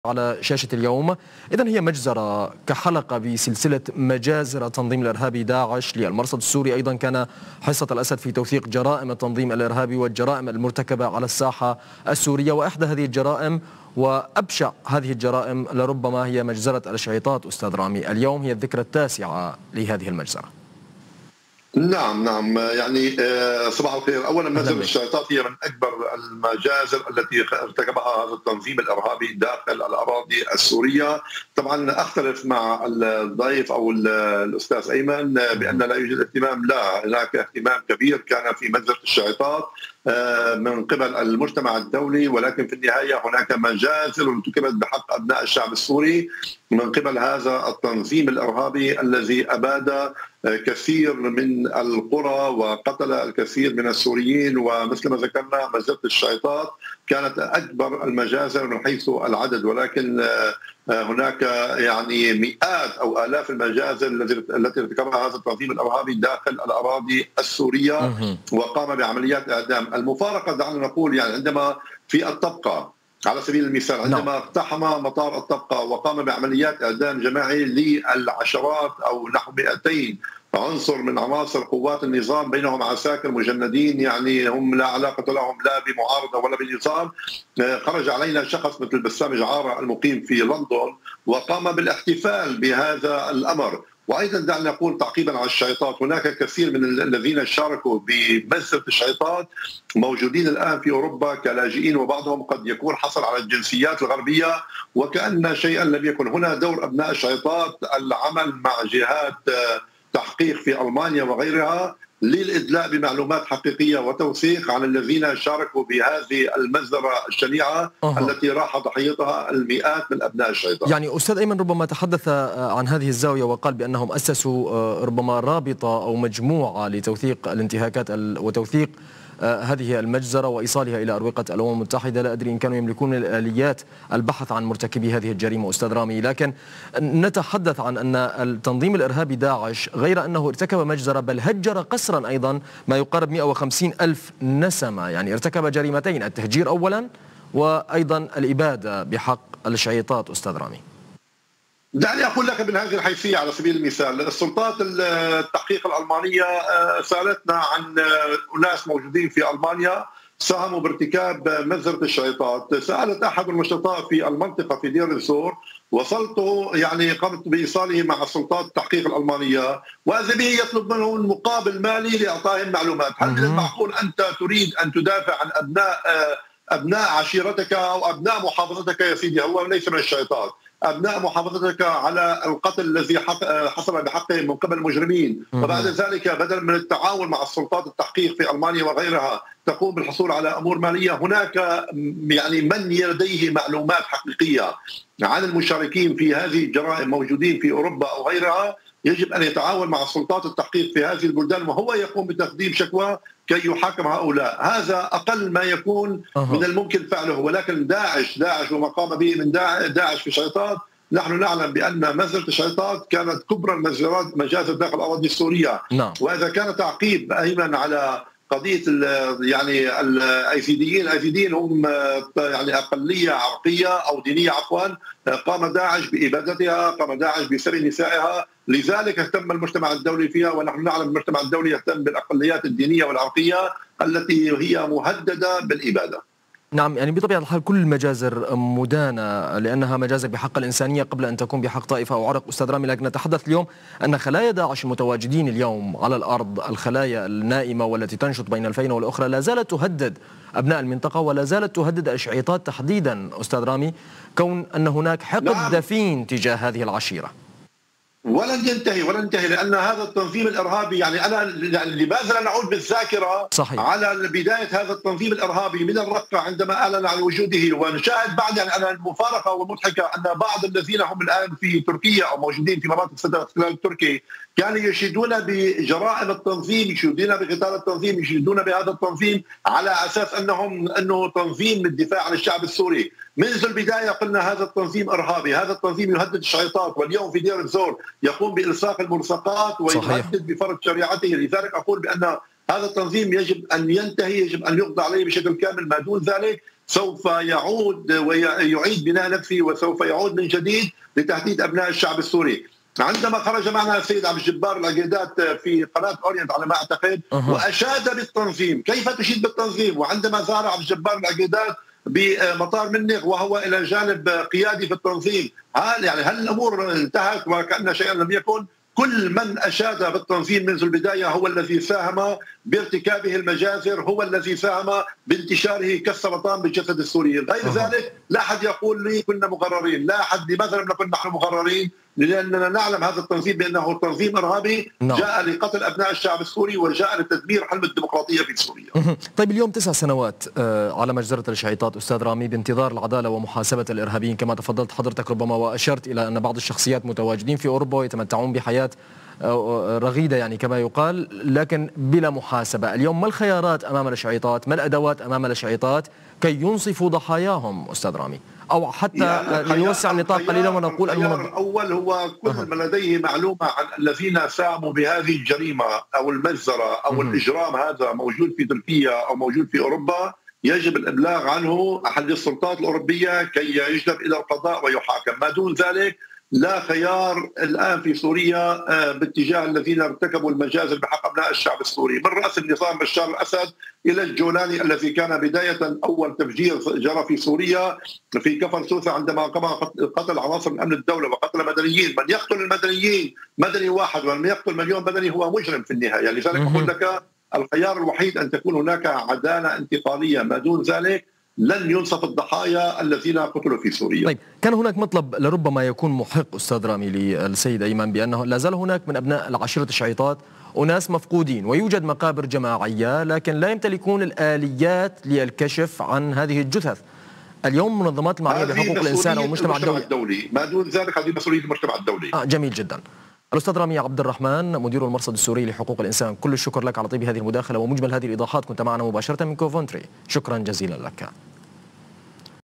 مجزرة الشعيطات من أكبر المجازر الدموية التي ارتكبها تنظيم "الدولة الإسلامية" من حيث العدد
مدير المرصد السوري: مجزرة الشعيطات من أكبر المجازر الدموية التي ارتكبها تنظيم "الدولة الإسلامية" من حيث العدد، وكان هناك اهتمام كبير بهذه المجزرة من قبل المجتمع الدولي، ولكن بالنهاية هناك مجازر كبيرة ارتكبت بحق أبناء الشعب السوري، على يد هذا "التنظيم الإرهابي"، الذي أباد الكثير من القرى وقتل وأعدم السوريين، ففي الطبقة اقتحم "التنظيم" المطار، ونفذ إعدام جماعي لنحو مئتي عنصر من عناصر قوات النظام بينهم عساكر مجندين، الذين هم لا علاقة لهم بالمعارضة ولا النظام، كما أن بعض الذين شاركوا في مجزرة الشعيطات هم متواجدين حاليا في أوربا كلاجئين وحصلوا على الجنسيات وكأن شيئا لم يكن.
هنا على أبناء الشعيطات لعب دور فعال مع جهات التحقيق في ألمانيا لإدلاء معلومات حقيقية وتوثيقات لمحاكمة هؤلاء الذين شاركوا في هذه المجزرة الشنيعة التي راح ضحيتها المئات وهذا "التنظيم " "الإرهابي" لم ينتهي، والبعض من عناصر "التنظيم موجودين في تركيا والمناطق الخاضعة لنفوذه في داخل الأراضي السورية، ويشيدون بالتنظيم، واليوم يقوم "التنظيم" بإلصاق ملصقات في شوارع دير الزور ويهدد بفرض شرعيته، لذا هذا التنظيم يجب أن ينتهي، وأن يقضي عليه، ما دون ذلك، سوف يعود في بناء نفسه مجدداً لتهديد أبناء الشعب السوري وتدمير حلم الديمقراطية في سوريا، الخيار الوحيد أن تكون هناك عدالة انتقالية ما دون ذلك لن ينصفوا الضحايا في سوريا، وهذه من مسؤولية المجتمع الدولي.